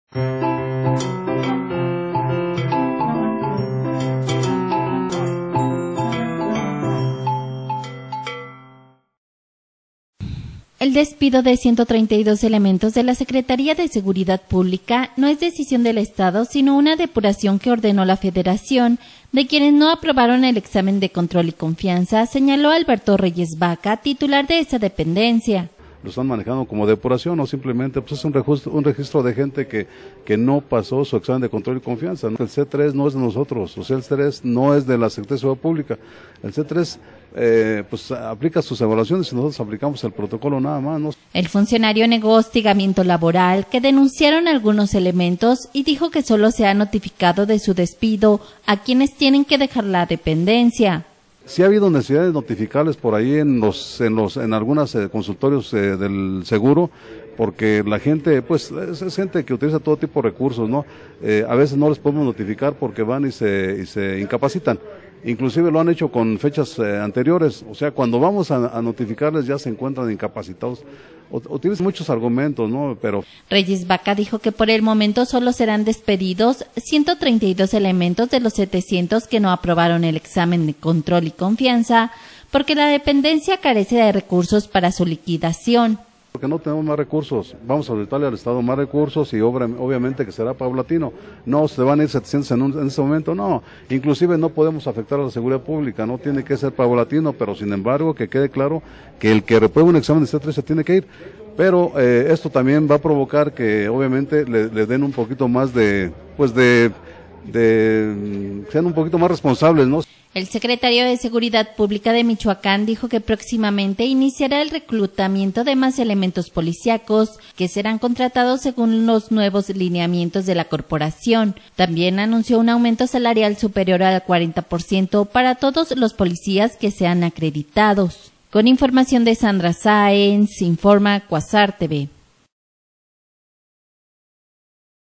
Federación ordenó depurara Policía de Michoacán: SSP
El despido de 132 elementos de la Secretaría de Seguridad Pública de Michoacán no es decisión del estado sino una depuración que ordenó la Federación de quienes no aprobaron el examen de control y confianza, señaló Alberto Reyes Vaca, titular de esta dependencia...